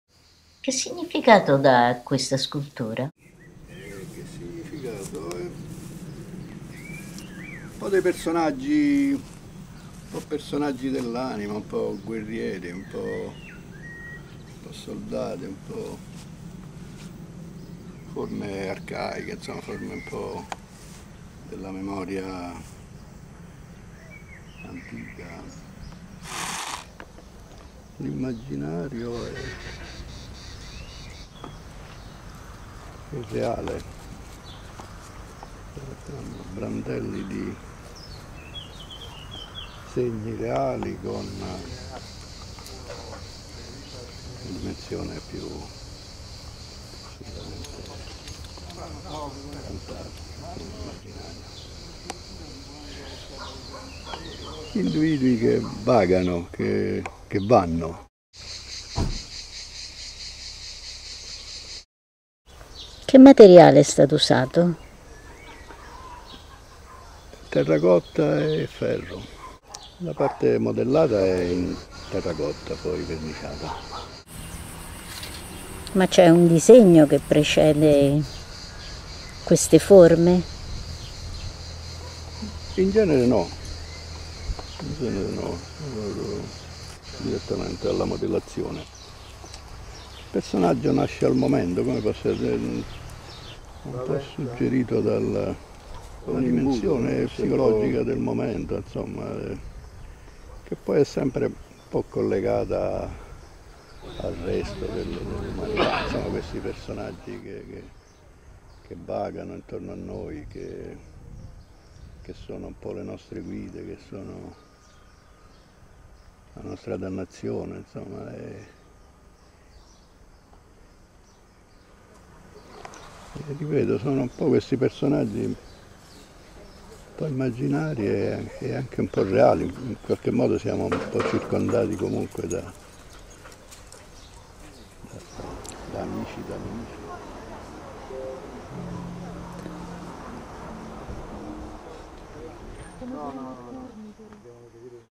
Arte nell'orto 2016, intervista a Biagio Iadarola
L' autore ci spiega il senso dell'opera "Incernierati"  installata nel museo a cielo aperto di Claudio Marini